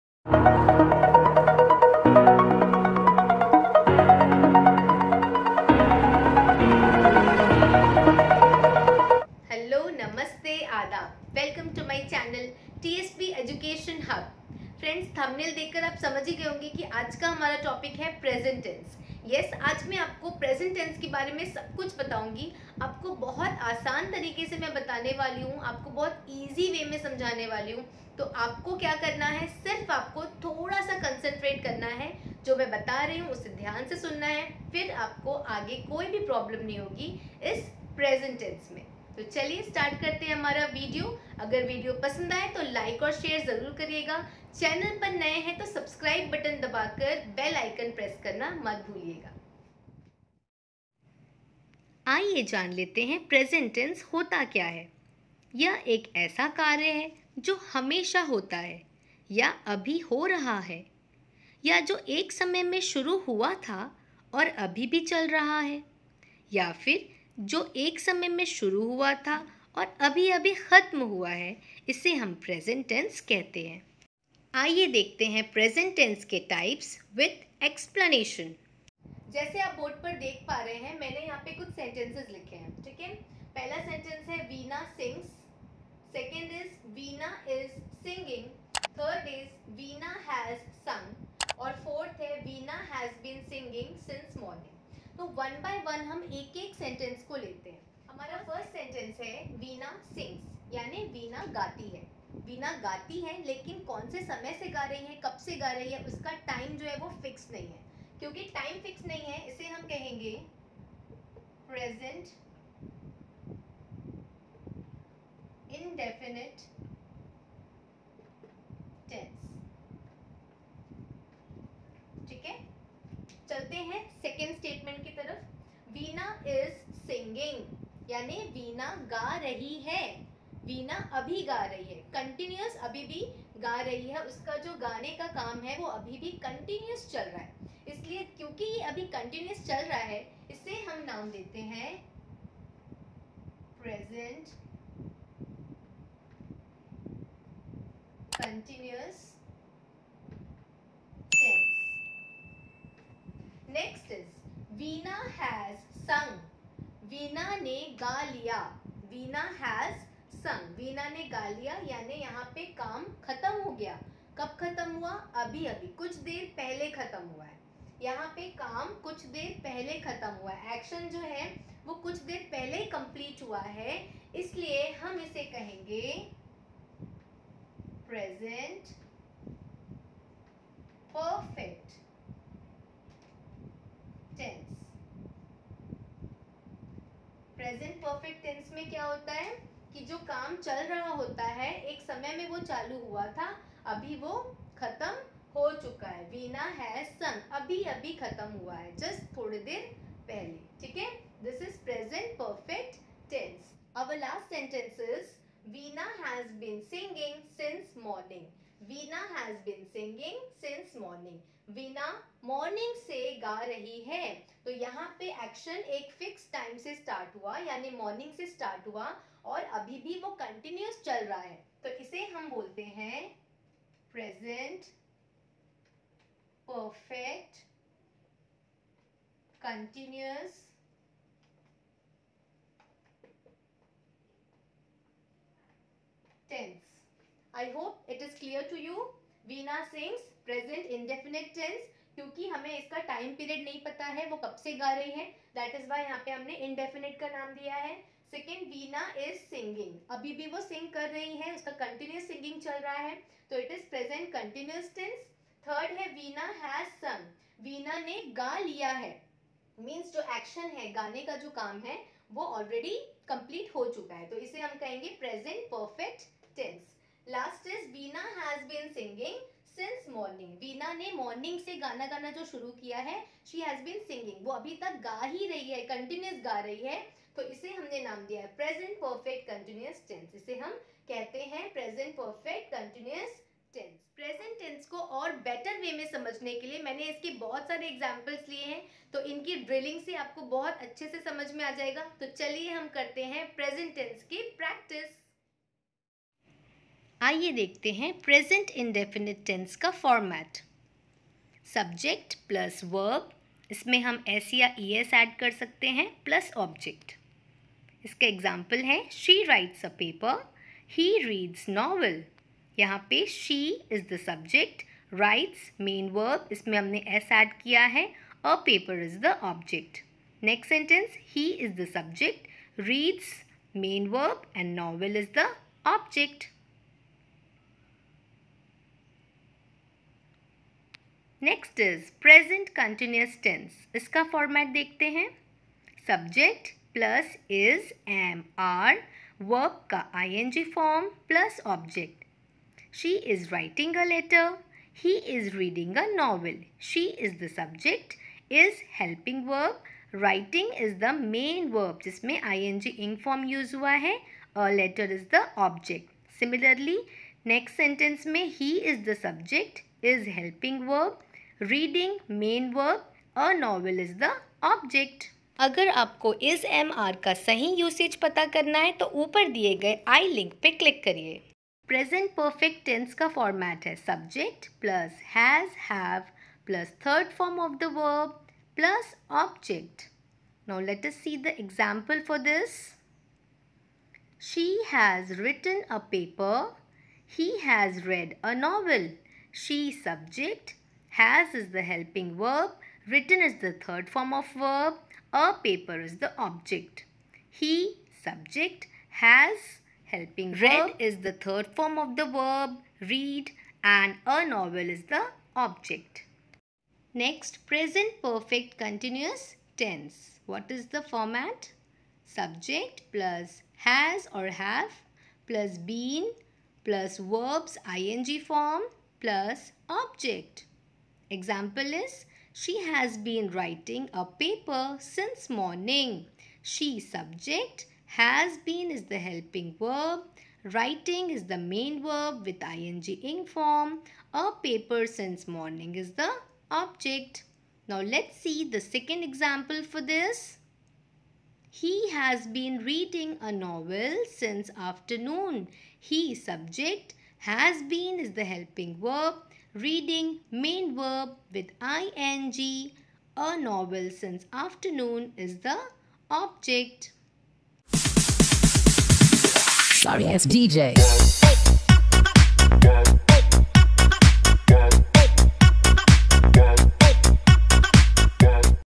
Present Tense // Tense // वर्तमान काल और वर्तमान काल के नियम // Present Tense Rules
This video contains all about present tense its rules and examples. .